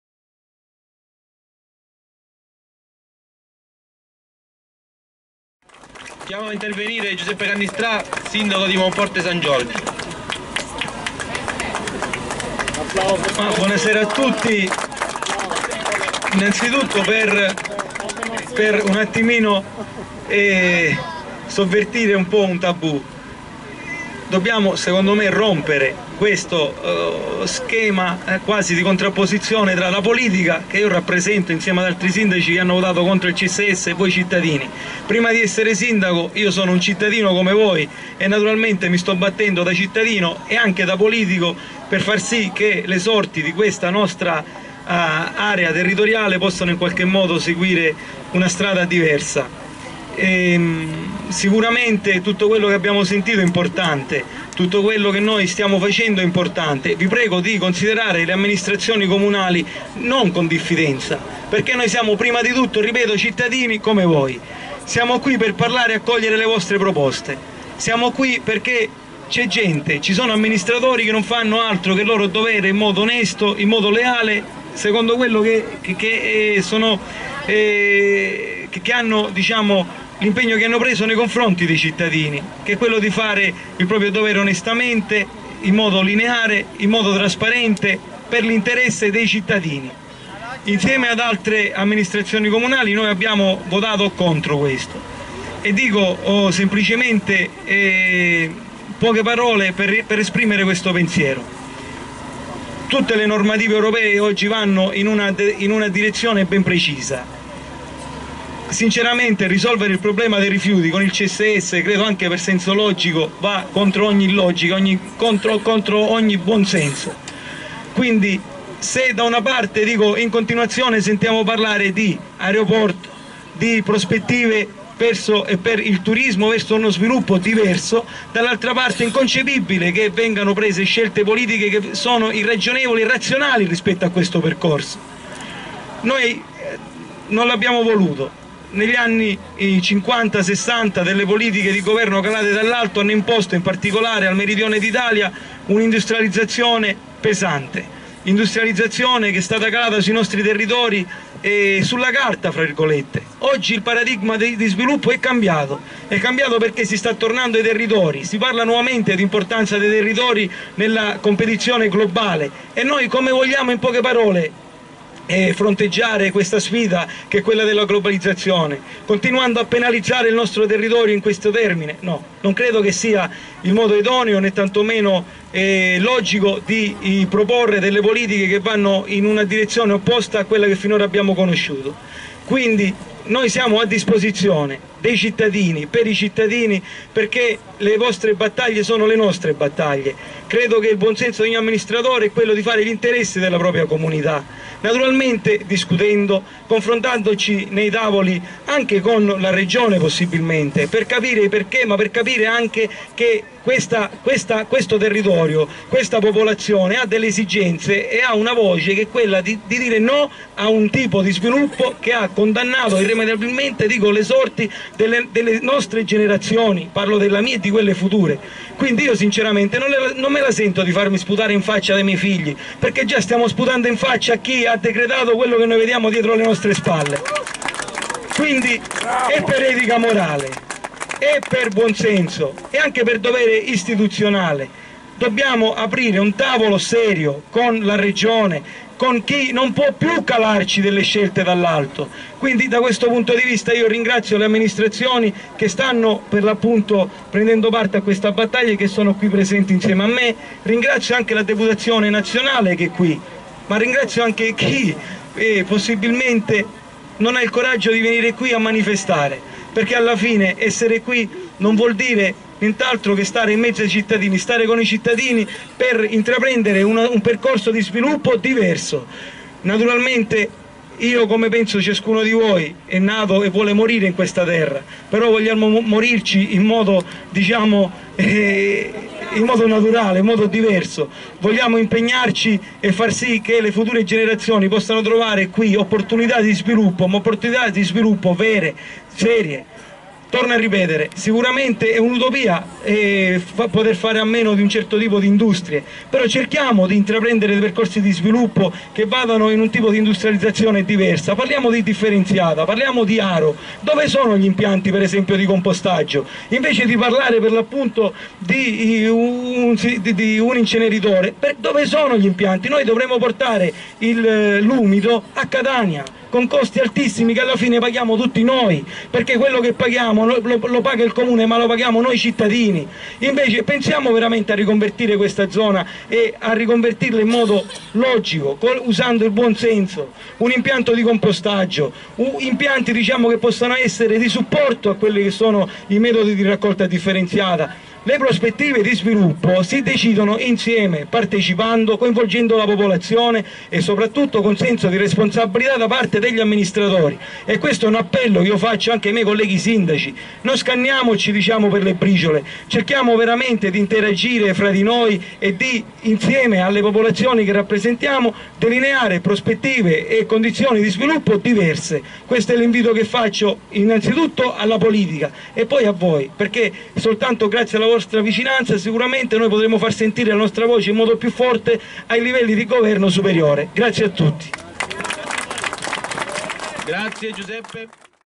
NO INCENERITORI valle del mela (ME) Giuseppe Cannistrà Sindaco di Monforte San Giorgio (ME) 5/6
NO INCENERITORI valle del mela (ME) GiuseppeCannistrà Sindaco di Monforte San Giorgio (ME)

[NoInceneritore] 5 risposte per smascherare le menzogne sul CSS, un progetto tragico per la centrale Edipower di S. Filippo del Mela (ME)